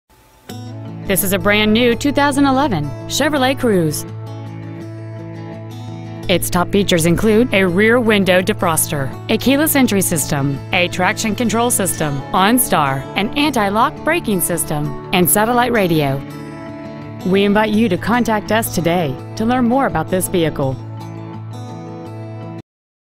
2011 Chevrolet Cruze Kent WA
Year: 2011
 Make: Chevrolet
 Model: Cruze
 Engine: 1.8L
 Trans.: Automatic
 Exterior: Ice Blue
 Miles: 50
 Interior: Jet Black

 2011 Chevrolet Cruze Auburn WA